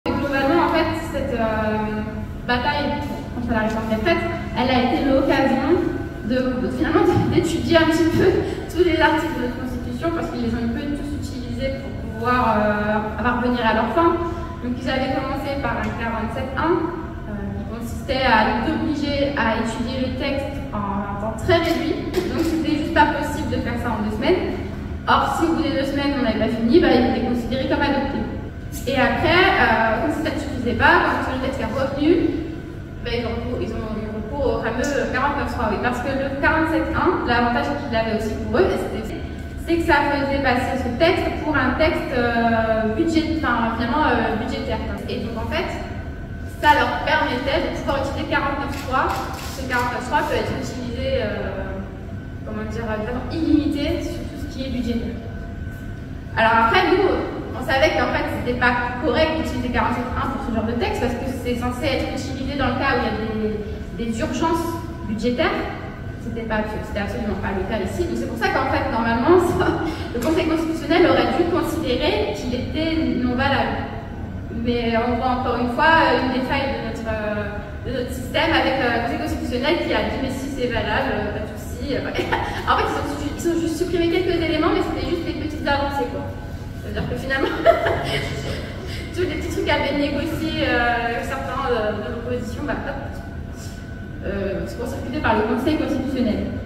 Ersilia Soudais - le 47-1 était anti-démocratique, le Conseil Constitutionnel aurait du censurer !
Lors de notre réunion publique à VarennesSurSeine hier, je dénonçais la stratégie anti-démocratique du gouvernement d'utilisation du 47-1 pour accélérer les débats, et le rôle trouble du Conseil Constitutionnel qui a mis le droit au service de la politique.